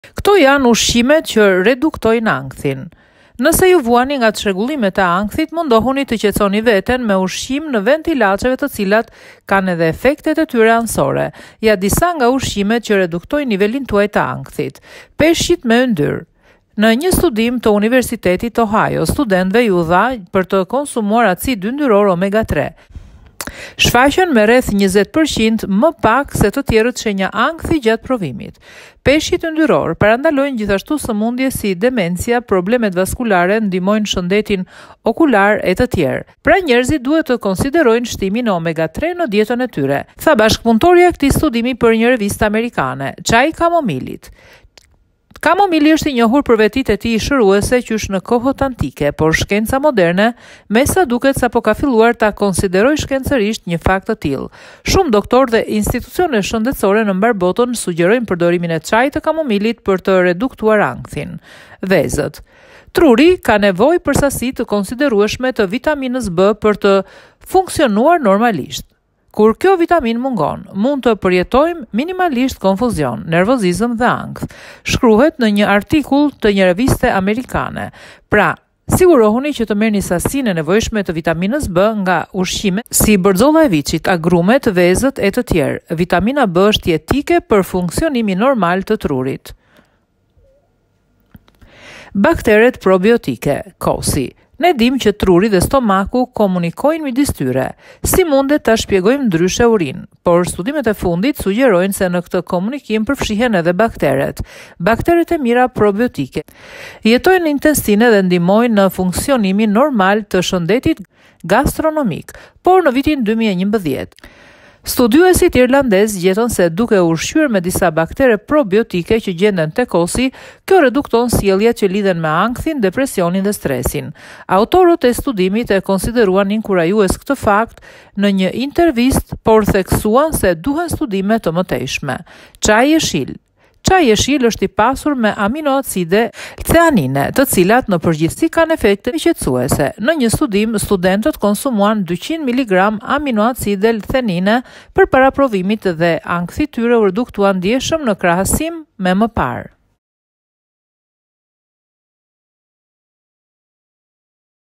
Ushqimet që reduktojnë ankthin
Nëse ju vuani nga çrregullimet e ankthit ,mundohuni të qetësoni veten me ushqim në vend të ilaçeve të cilat kanë dhe efekte anësore.